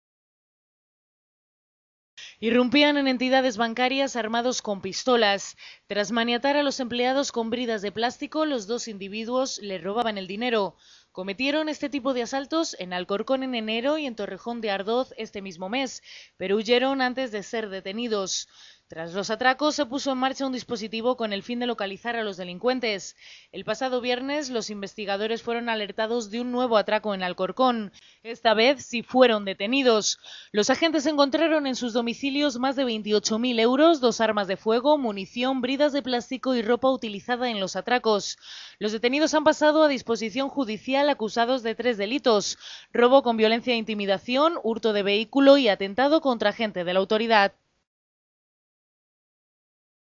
Dos detenidos por robos en bancos
Irrumpían en entidades bancarias armados con pistolas. Tras maniatar a los empleados con bridas de plástico, los dos individuos les robaban el dinero. Cometieron  este tipo de asaltos en Alcorcón en enero y en Torrejón de Ardoz este mismo mes, pero huyeron antes de ser detenidos.